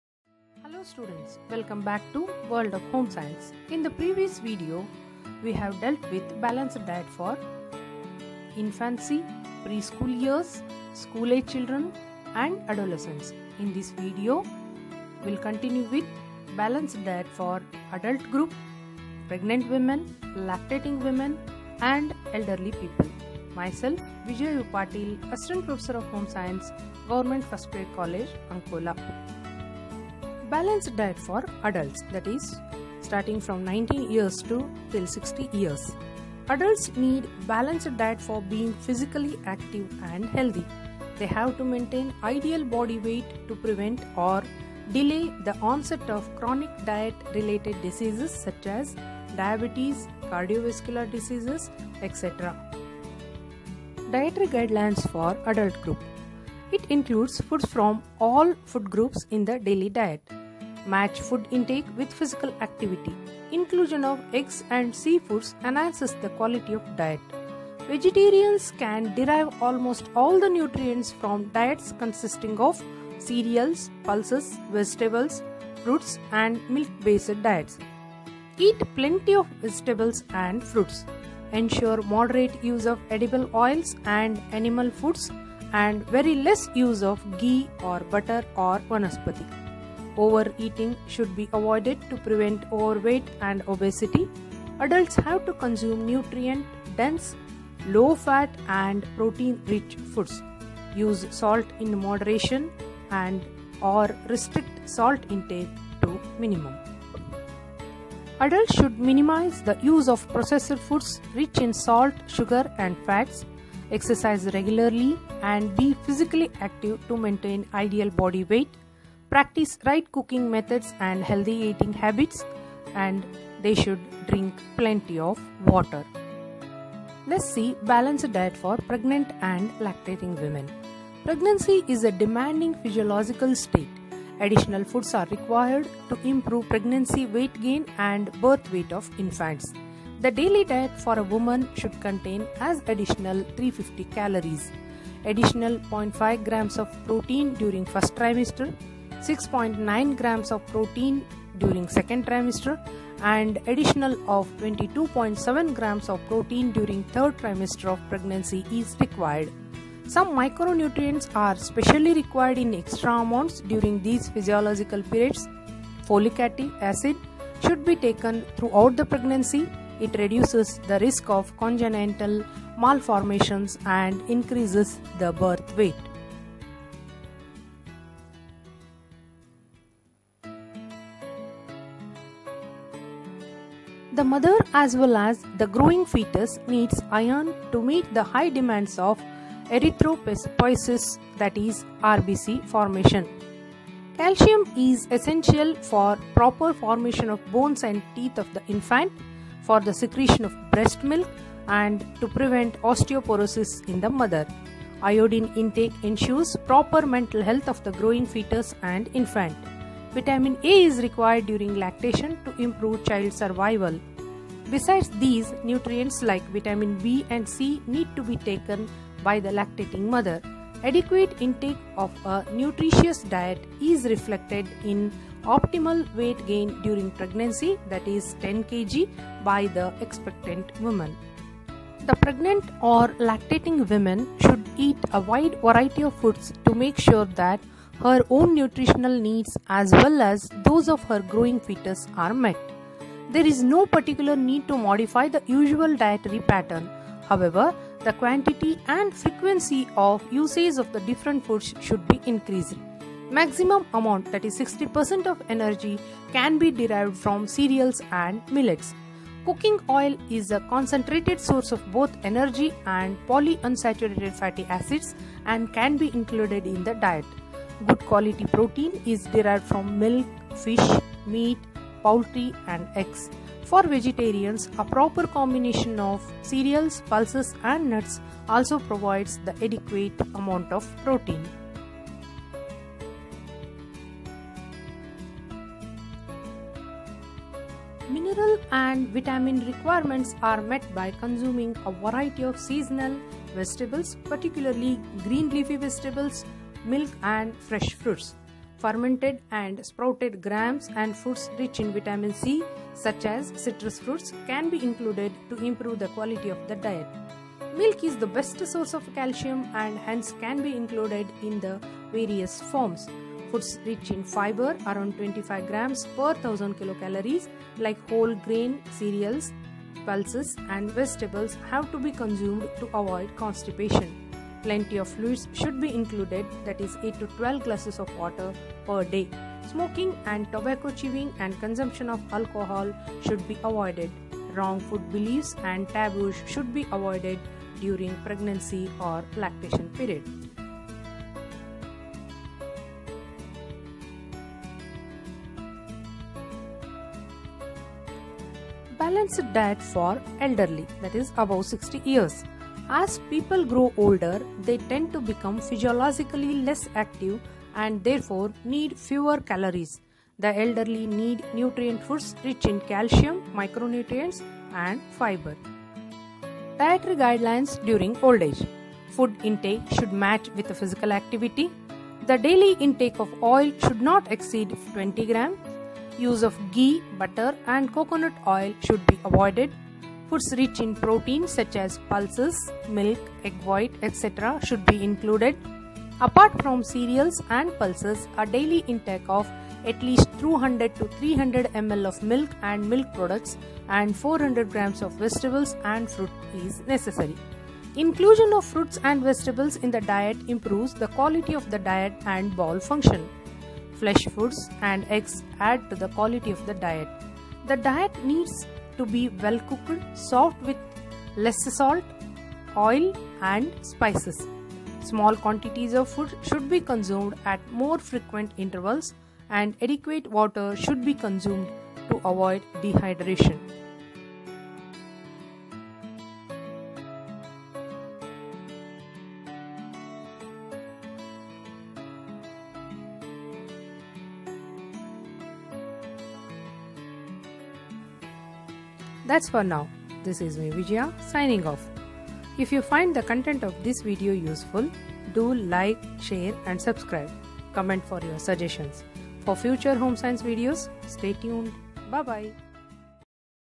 Meal Planning (Balanced Diet for Adults, pregnant and lactating women)(Class 12/ PUC II/ BA/BSc)
Home Science (Food Science,Nutrition and Dietetics)